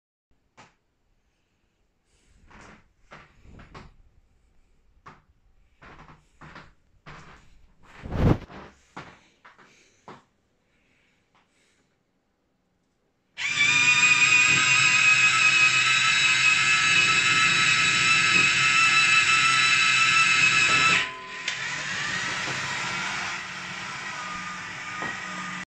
Project 3D Printed Downrigger Test 2
I have been experimenting with different, designs, i think i like this the most, simple, one single bolt to fasten the spool, boom, and later the electronics cover..